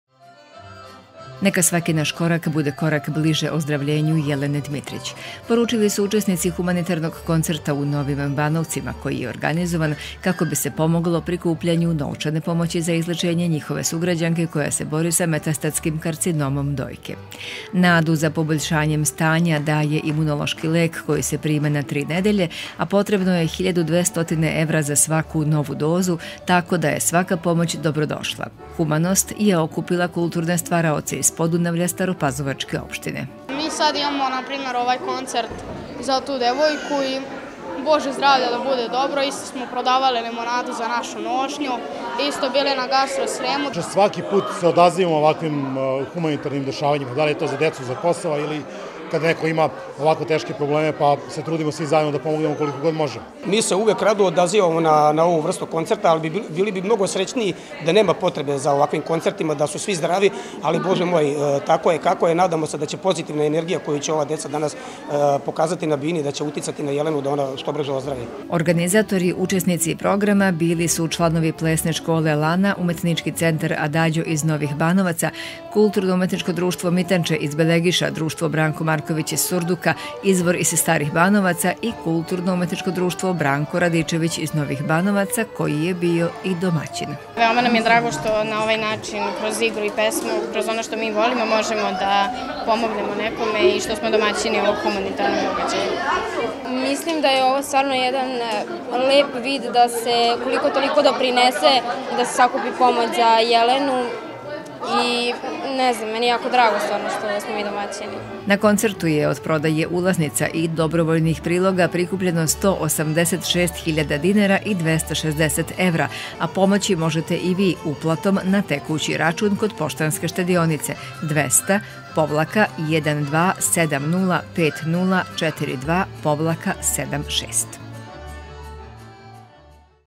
Humanitarni koncert za Jelenu Dmitrić
U Domu kulture u Novim Banovcima  u subotu je održan  humanitarni koncert "Za Jelenu", koji su kulturni pregaoci iz  Podunavlja zajedno organizovali, kako bi pomogli prikupljanju novčane pomoći za izlečenje Jelene Dmitrić. Na koncertu je od prodaje ulaznica i dobrovoljnih priloga prikupljeno 186.000 dinara i 260 evra. Pomoći možete i vi uplatom na t.r. kod Poštanske štedionice: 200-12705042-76.